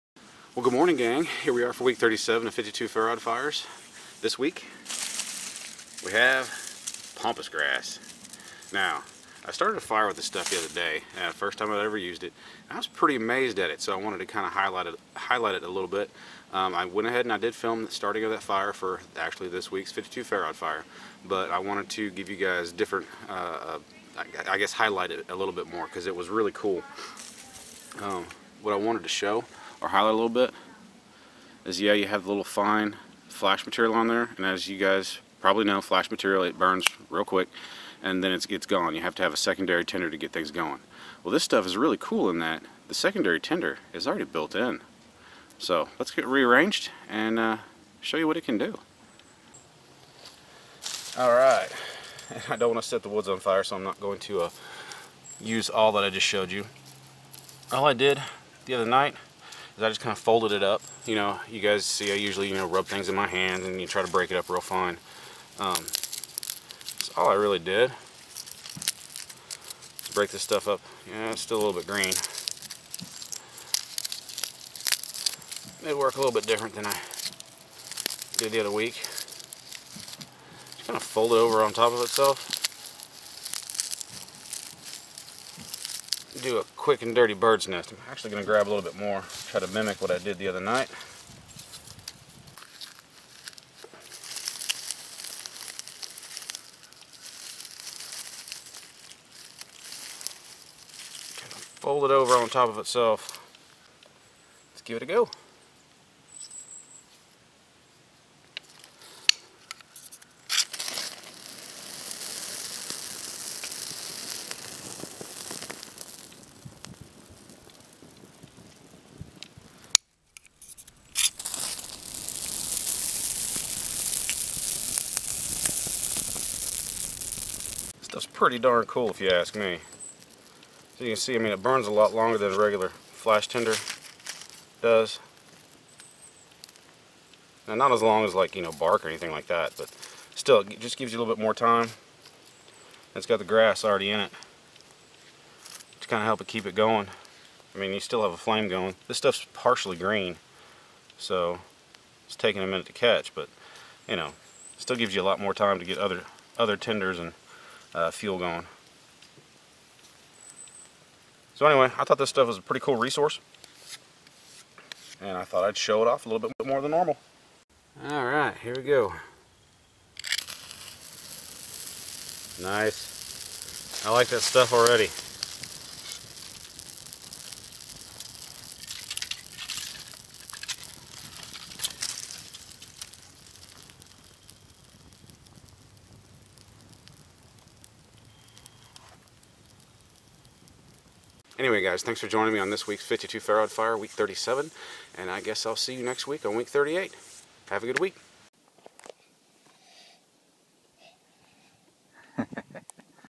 52 Ferro Fires Week 37 - Pampas Grass
This week's 52 Ferro Fire tinder is Pampas Grass.  Trust my guys, this one is a very cool resource.